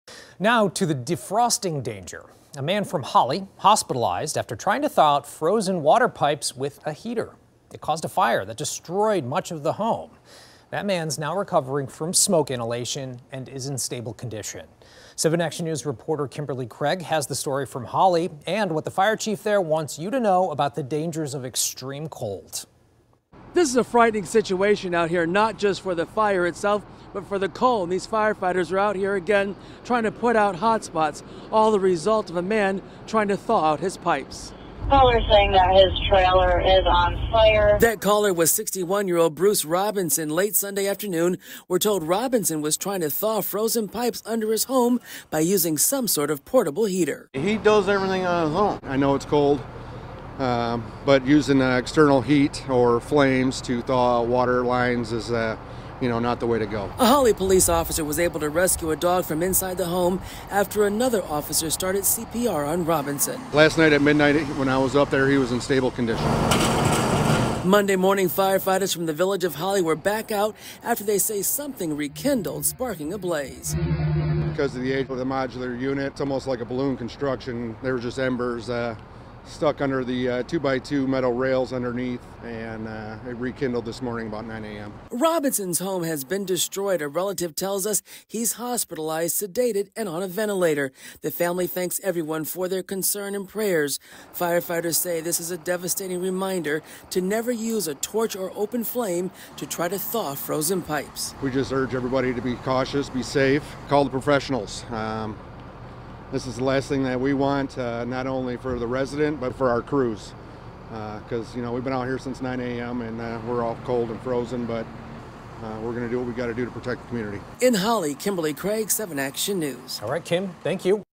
Home destroyed by fire after Holly man tries to thaw frozen pipes with portable heater
A Holly man is hospitalized, suffering from smoke inhalation and hypothermia after fire officials say he tried to use a portable heater to thaw frozen pipes under his home.